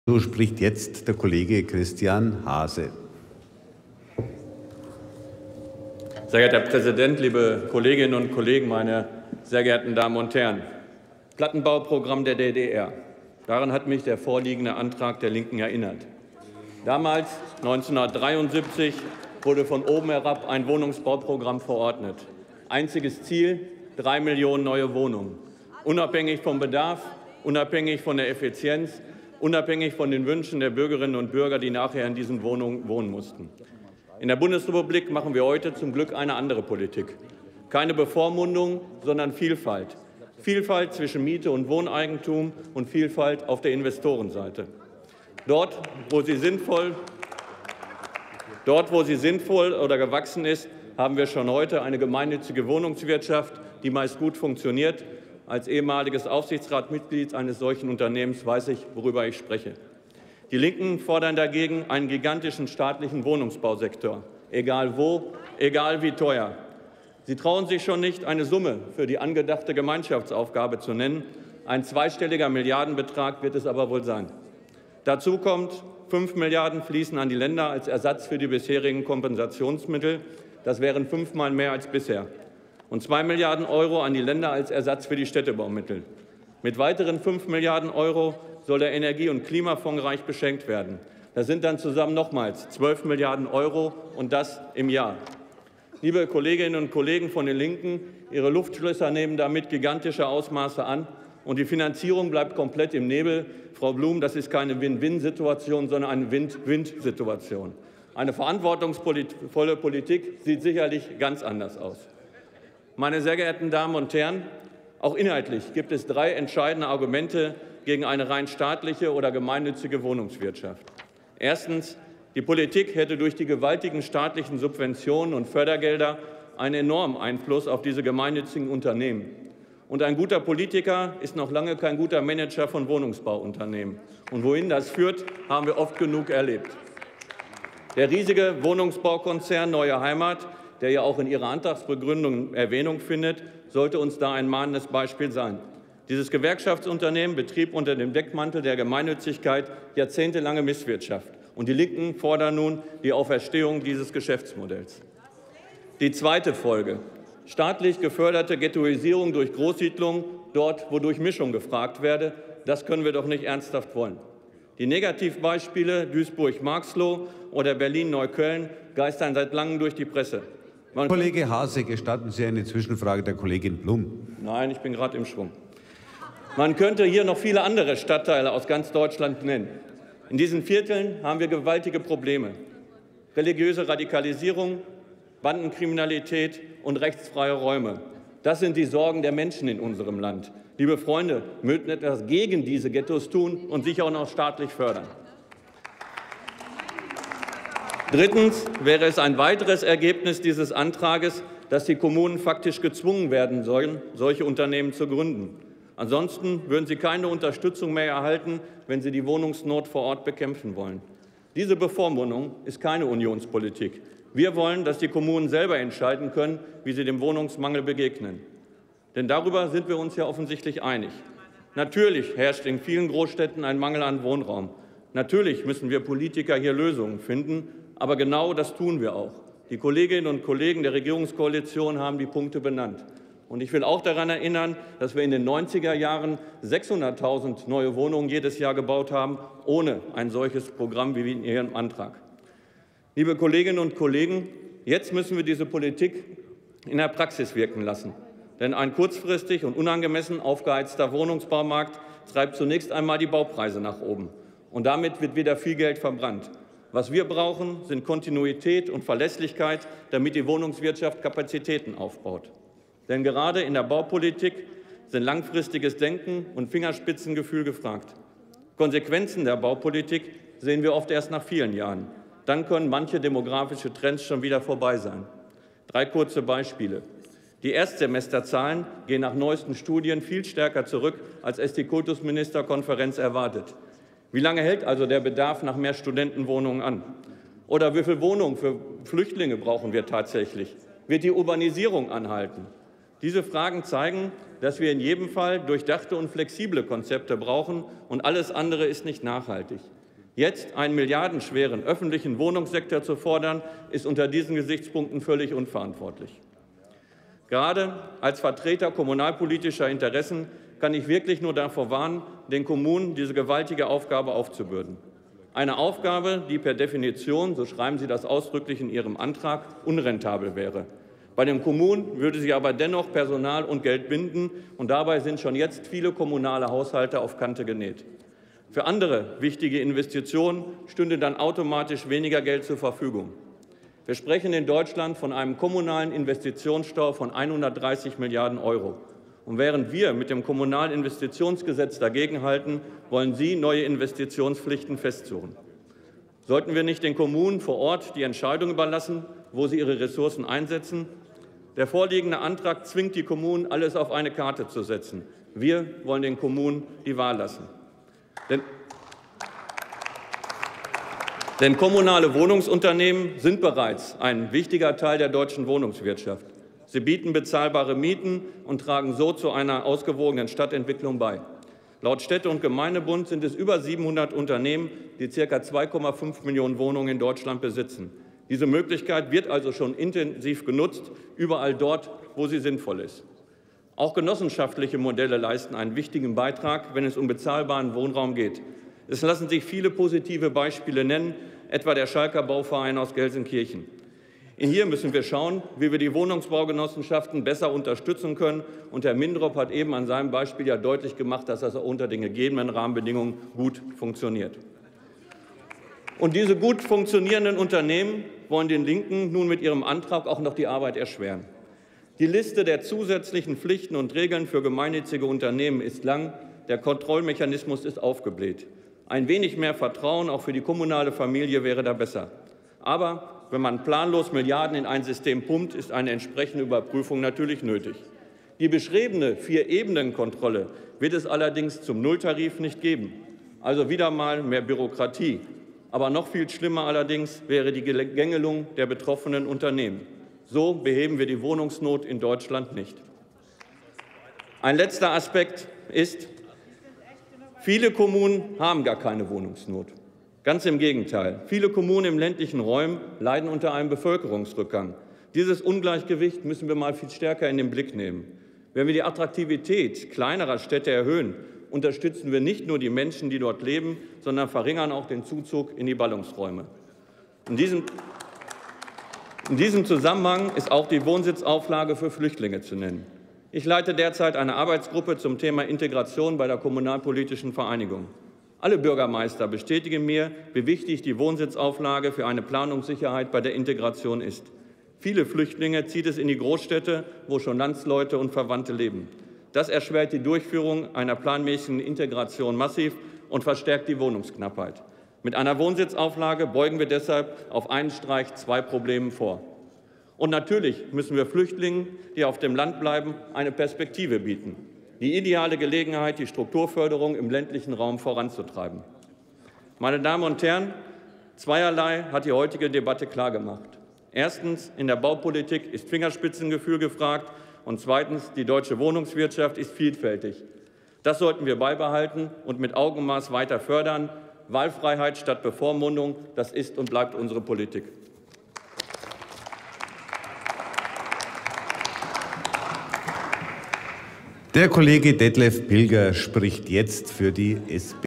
Christian Haase: Aktionsplan für gemeinnützige Wohnungswirtschaft [Bundestag 14.04.2016]
Bundesweiten Aktionsplan für eine gemeinnützige Wohnungswirtschaft auflegen
Drucksache 18/7415

Die neue Wohnungsgemeinnützigkeit – Fair, gut und günstig wohnen
Drucksache 18/8081

18. Deutscher Bundestag
164. Plenarsitzung
Tagesordnungspunkt: Aktionsplan für gemeinnützige Wohnungswirtschaft
Redner: Haase, Christian, CDU/CSU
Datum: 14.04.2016
Plenarprotokoll:
UVID:6753269